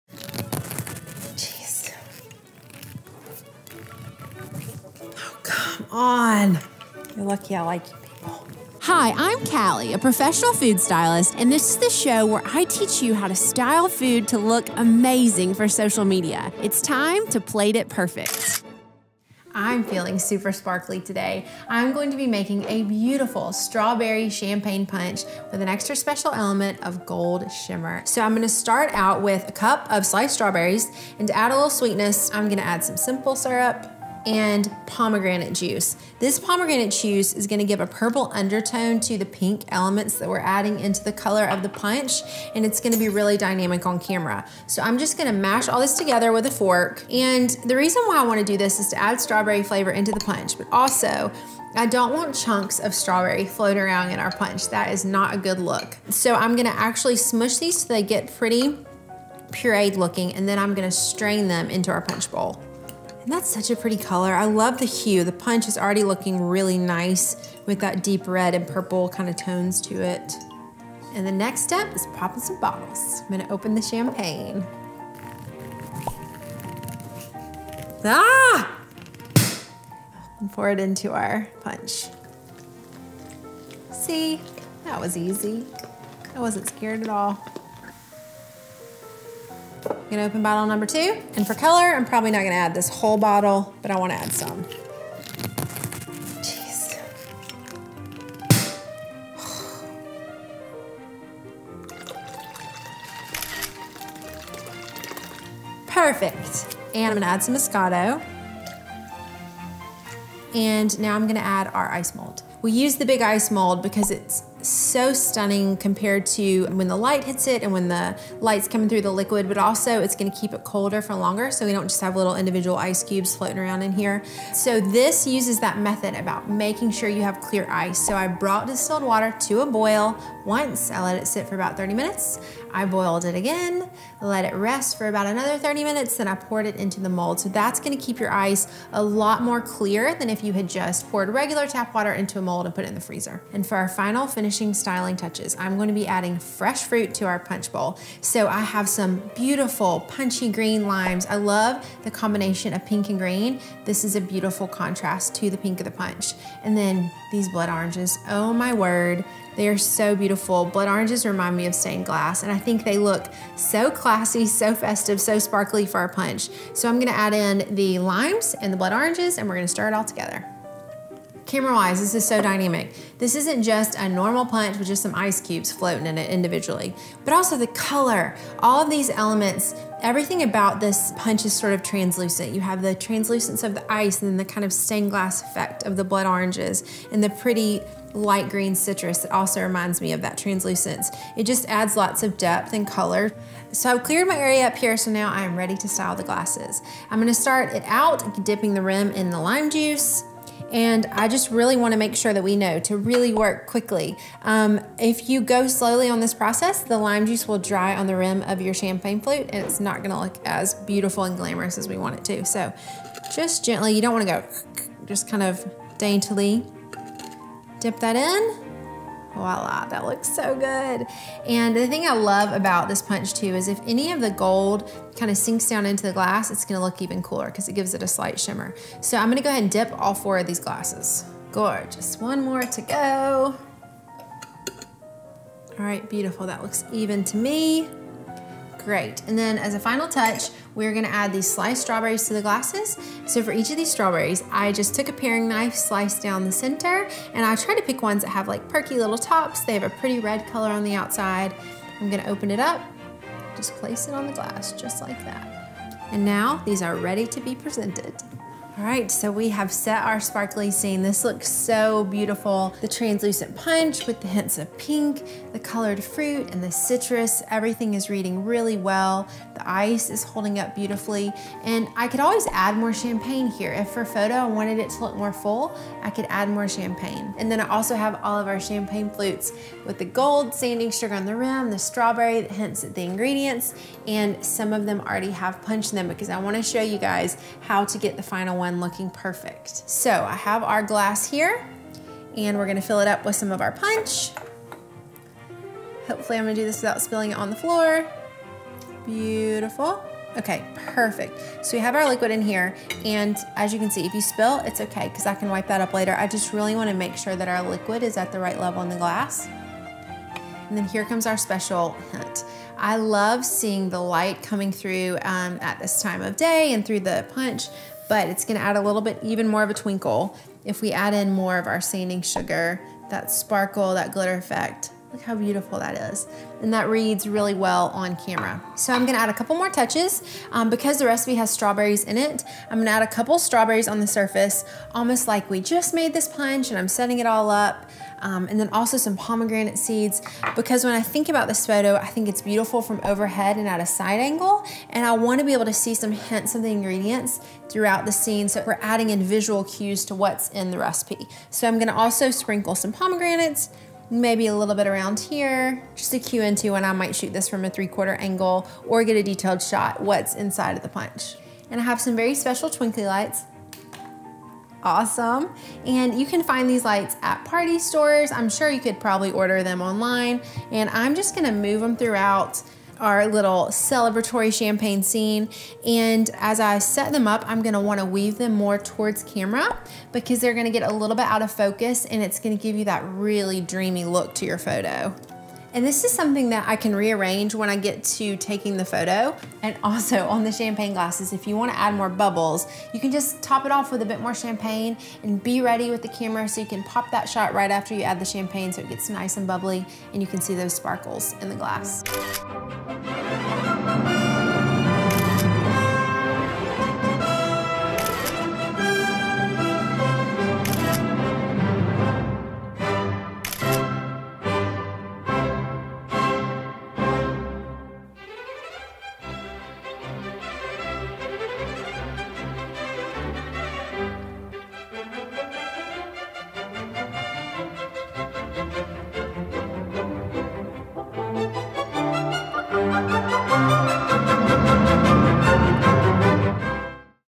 Extravagant Champagne Punch: Plate It Perfect
On this episode of Plate It Perfect, Callie shows you how to make a delicious champagne punch. See how she styles it to make the perfect party photo!

Styling Tips:
+ Use ice molds
+ The more fresh fruit, the better
+ Extra details like gold sanding sugar go a long way in making the punch sparkle!

Recipe:
+ Ingredients
1 gallon distilled water
2 blood or regular oranges, sliced lengthwise
2 limes, sliced & zest of 1 lime
1 cup sliced strawberries, plus additional whole berries to garnish glasses
3/4 cup Moscato
2 bottles sparkling wine
1 Tbsp simple syrup
1/2 cup pomegranate juice
Gold sanding sugar, to rim glasses

1. Prep ice: Boil distilled water. Let rest for 30 minutes and boil again. Allow to cool, then pour into ice mold. Freeze overnight.
2. Stir together simple syrup and pomegranate juice and strawberries until combined; lightly mush. Strain through a strainer and add the juice to punch bowl. Add Moscato and sparkling wine and stir to combine.
3. Put ice mold in punch bowl. Add sliced limes and oranges.
4. Rim glasses: Dip champagne flutes in fresh lime juice. Dip again in sanding sugar. Fill with punch; add a pinch of glitter to each glass. Add a slice strawberry to the rim of the glass and enjoy.